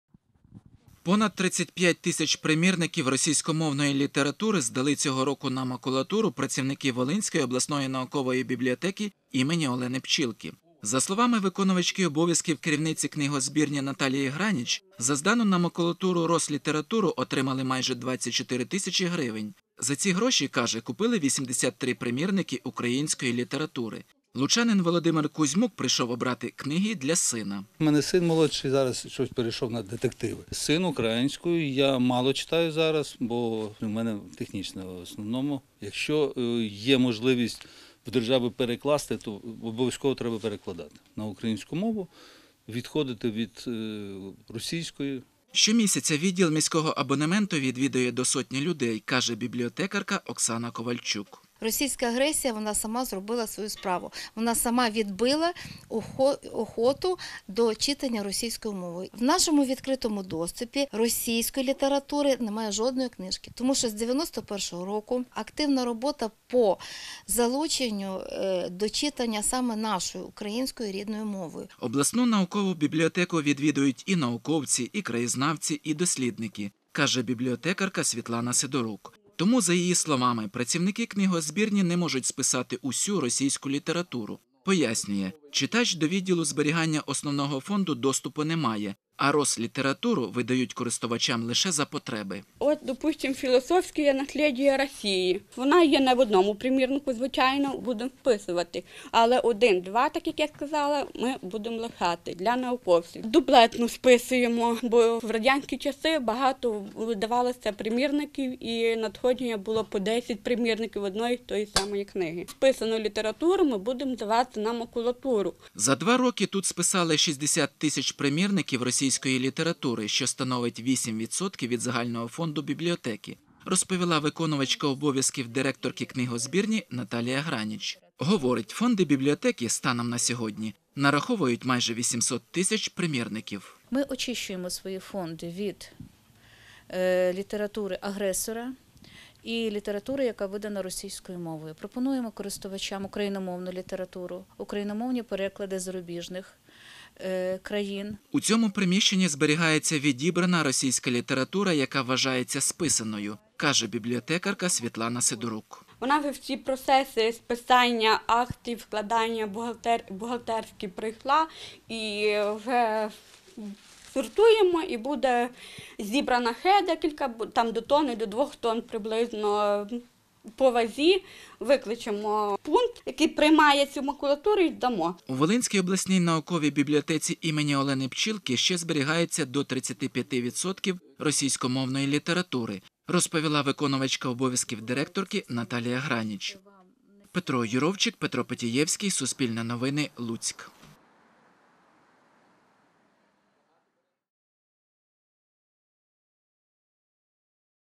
Понад 35 тисяч російських книг з фондів волинської наукової бібліотеки здали на макулатуру
Понад 35 тисяч примірників російськомовної літератури здали цього року на макулатуру працівники Волинської обласної наукової бібліотеки імені Олени Пчілки. За словами виконувачки обов’язків керівниці книгозбірні Наталії Граніч, за здану на макулатуру російську літературу отримали майже 24 тисячі гривень.  За ці гроші купили 83 примірники української літератури.